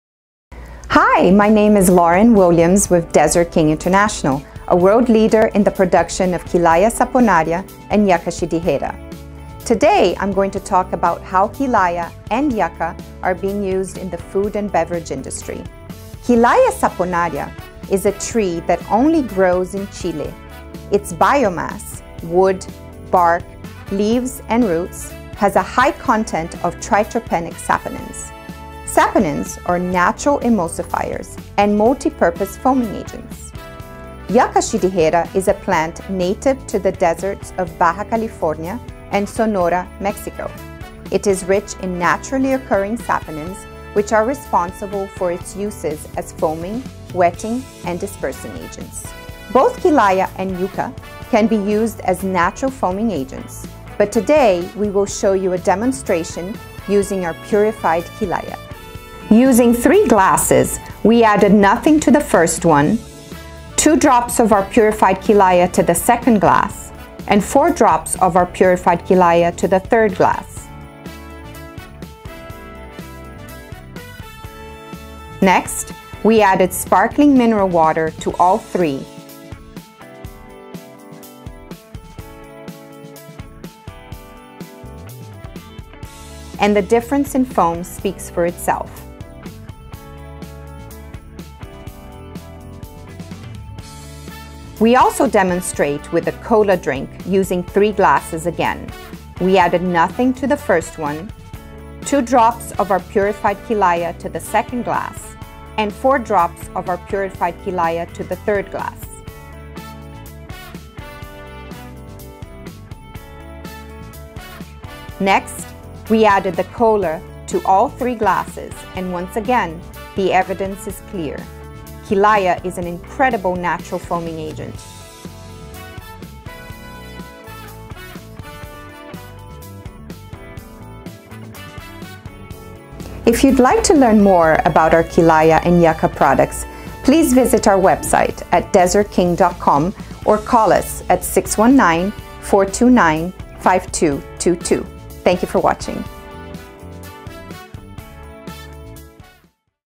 Desert King Quillaja & Yucca Video F&B 2019
How Quillaja & Yucca are used in the Food & Beverage industry